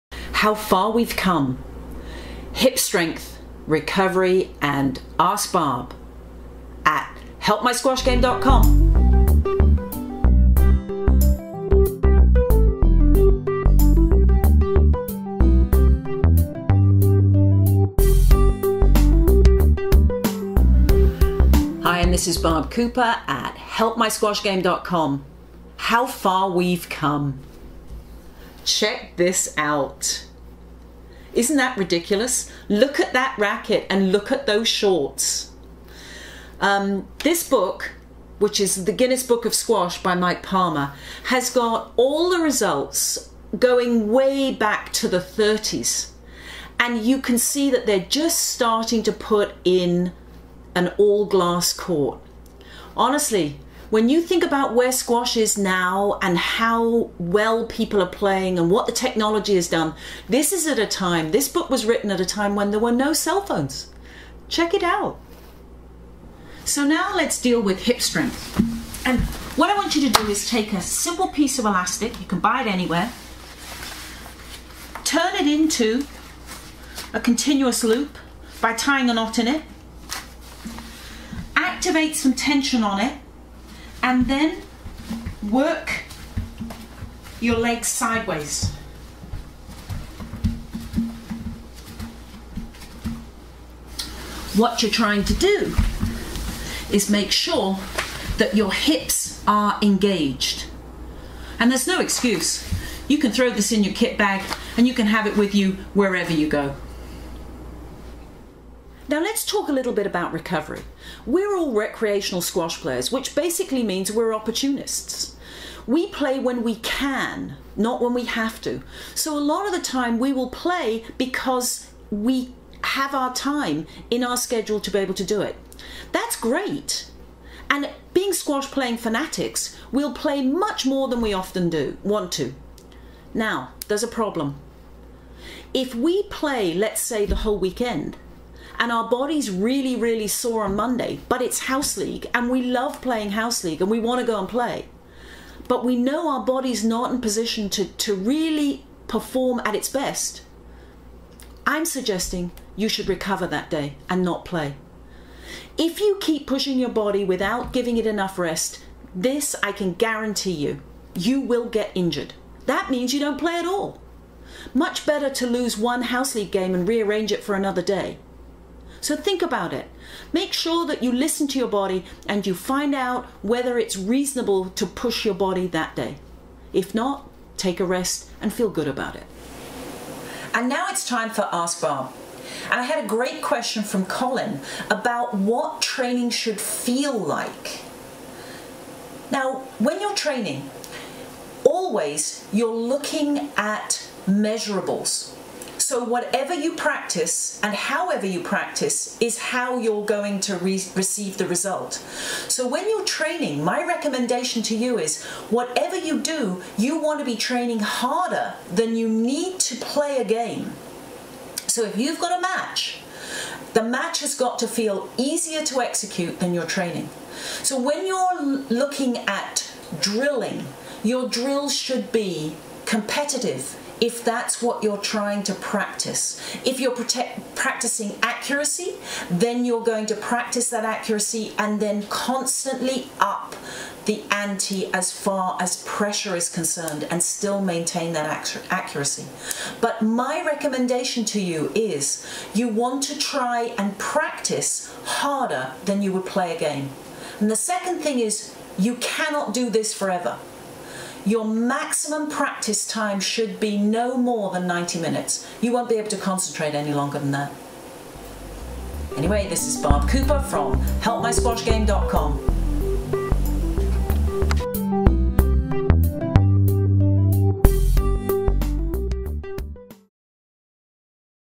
Help My Squash Game Show 34 Squash Tips with Barb Cooper
How far we have come the Guiness Book is Squash written by Mike Palmer
shows the history and evolving of the game of squash.

Hip Strength using a wide elastic band around the ankles.

Ask Barb, Colin asks "what should squash training feel like?"